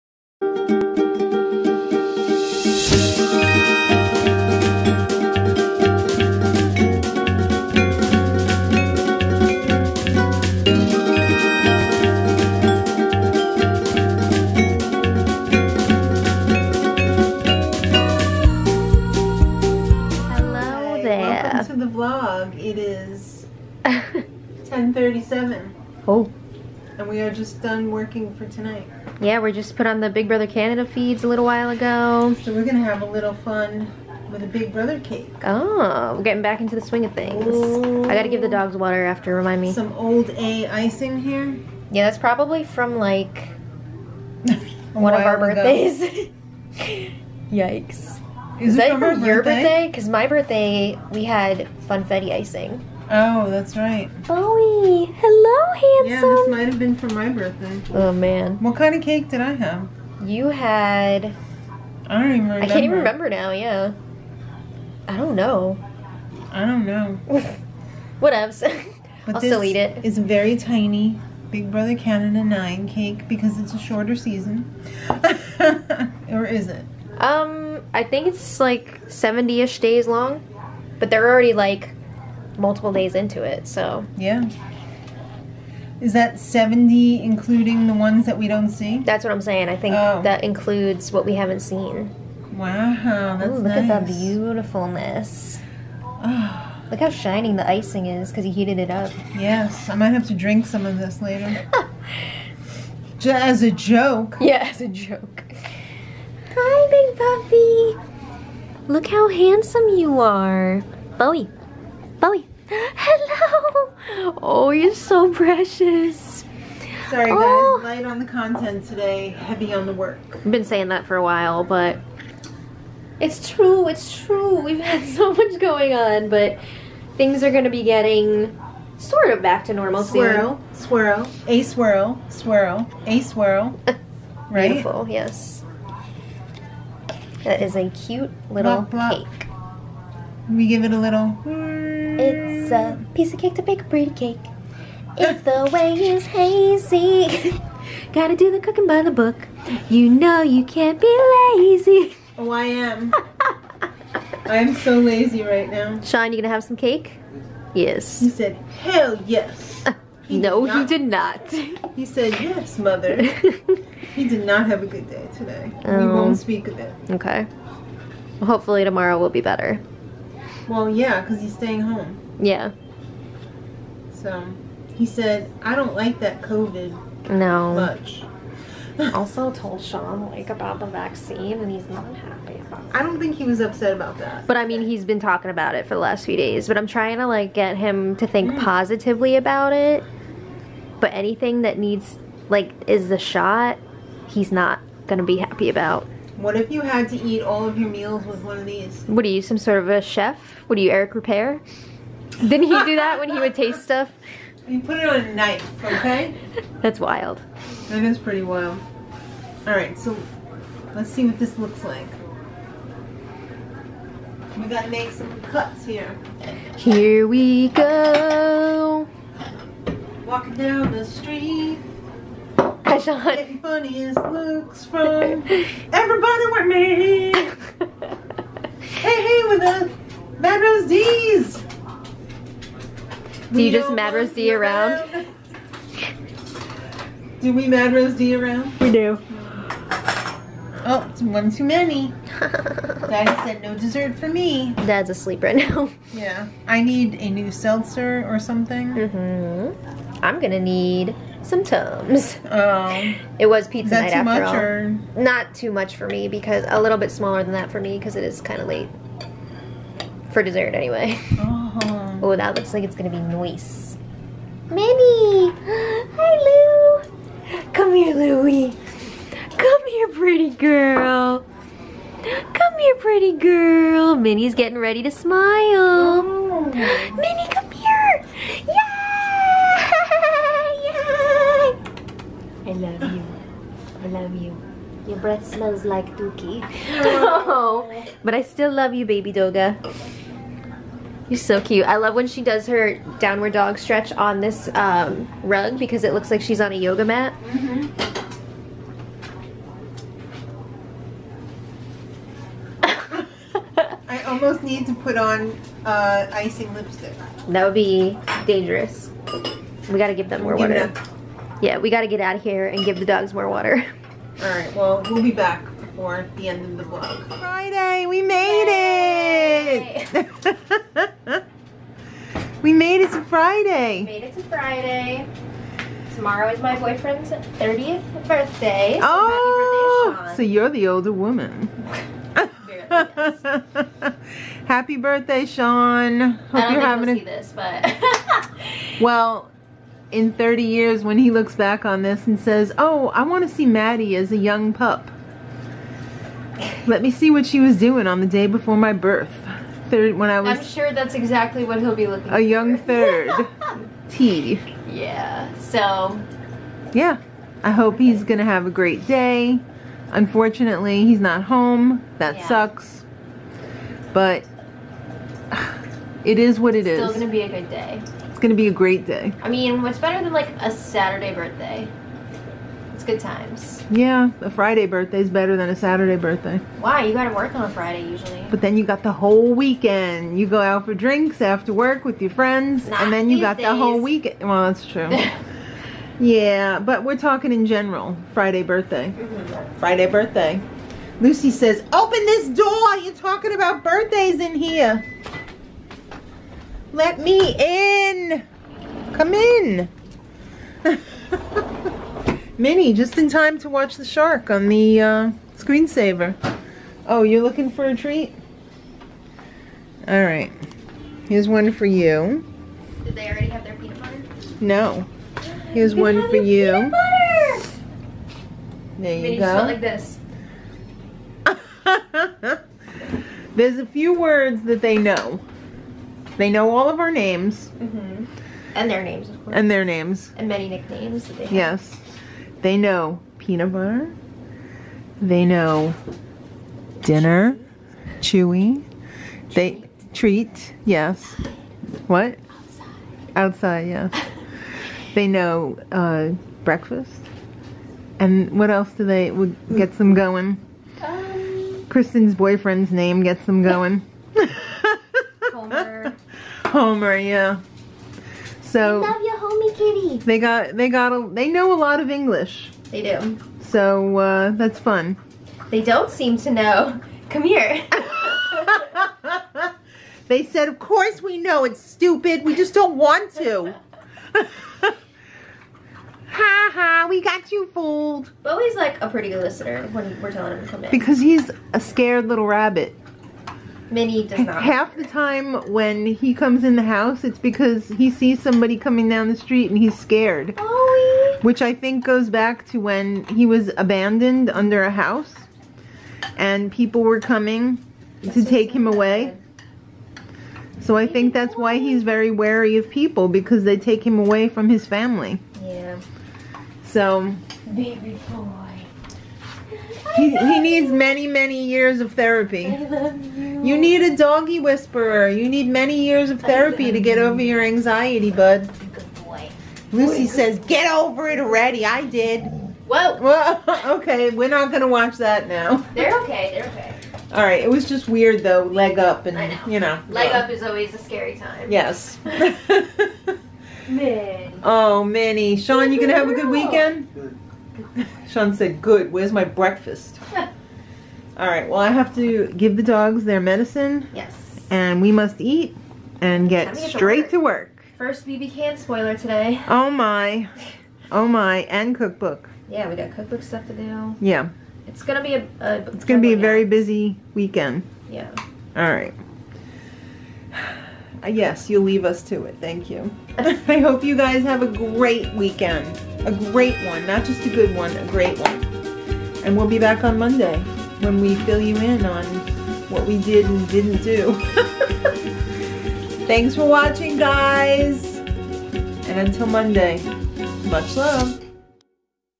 Here We Go Again!
Here We Go Again! (03/04/21)
BBCan9 started again! I guess we'll be hunkered down for the next 70 days lol! Thanks for being awesome - we much love you!

ON THIS DAY:
1 Year Ago:
2 Years Ago:
3 Years Ago:
4 Years Ago: